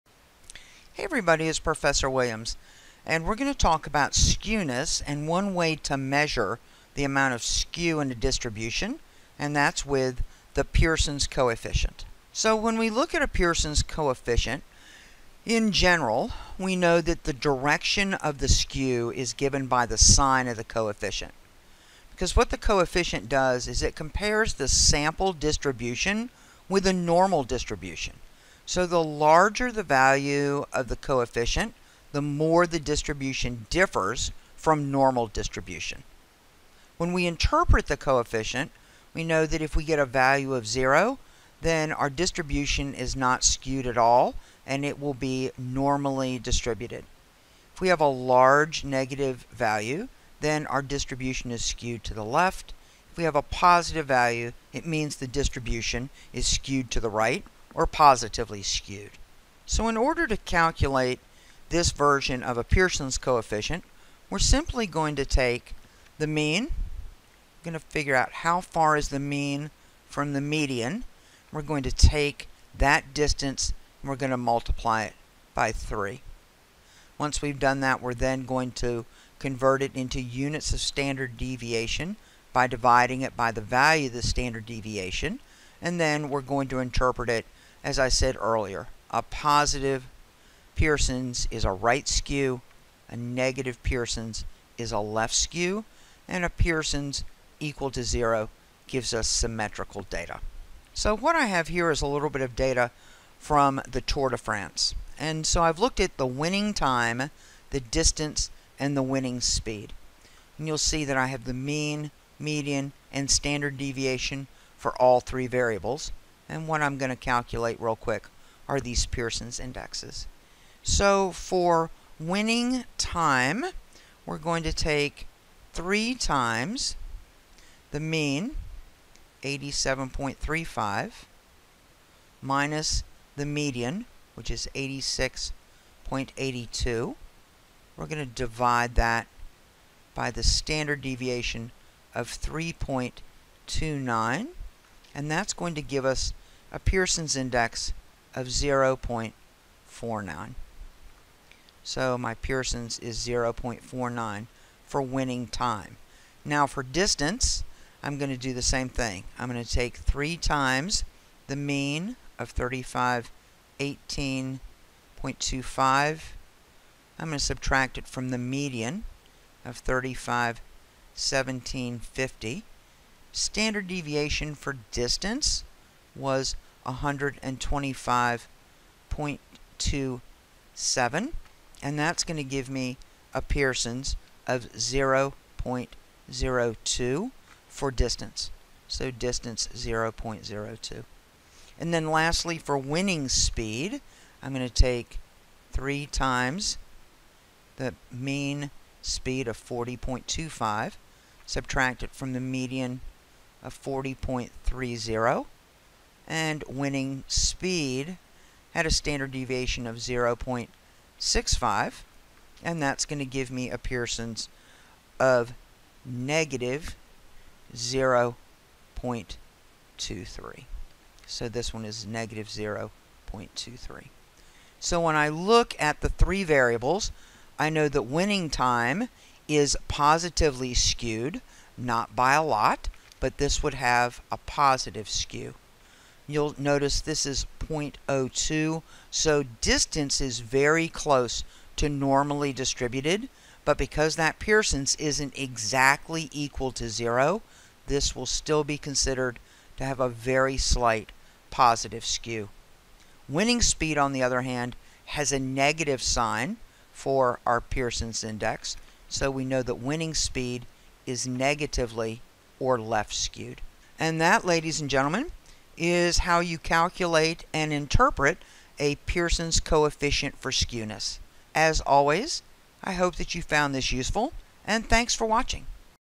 Statistics: Skewness and the Pearsons Coefficient
This video explains how to calculate and interpret the Pearsons Coefficient of skewness to determine the direction and degree to which a distribution is skewed.